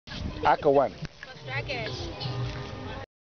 KIDS AND SOCCER IN GHANA II
Kids play at Amosima, an ancient town in the Central region of Ghana.